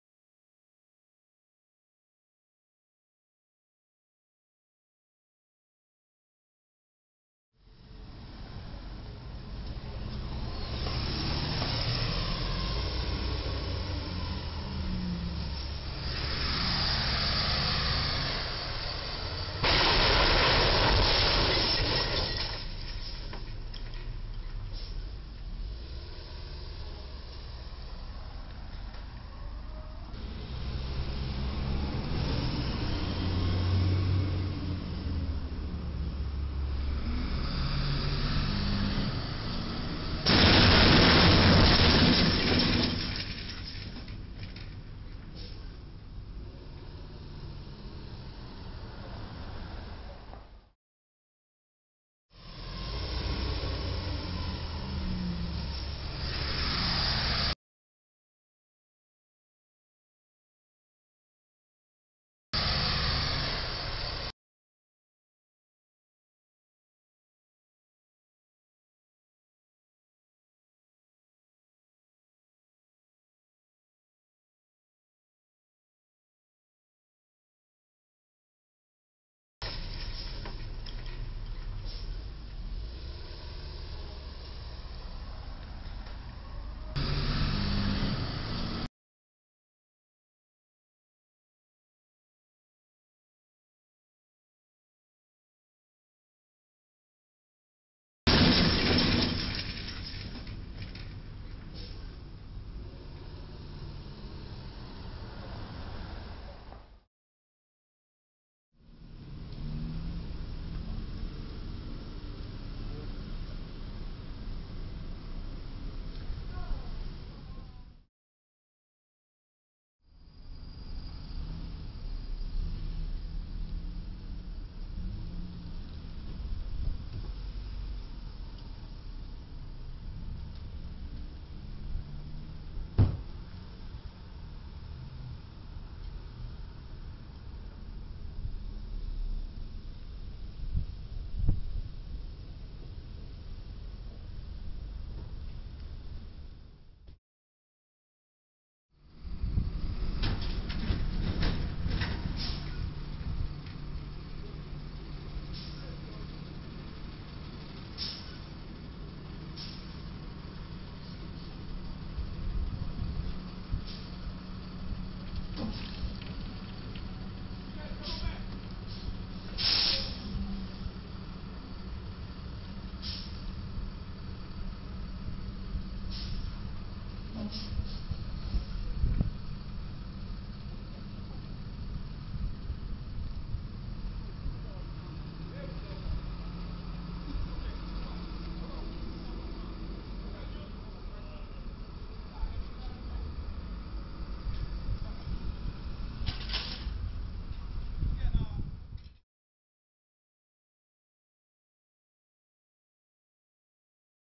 New Year's Eve Penske Peel at the 11foot8 bridge
On New Year's Eve 2017, the bridge got another holiday feast, peeling the top off another Penske rental truck. The driver left the scene of the accident after a few Minutes and left the busted truck sitting there in the middle of a busy city road without hazard lights on or any attempt at securing the accident. Later, the Police had to extract the truck from under the bridge. Not sure what the deal was with that.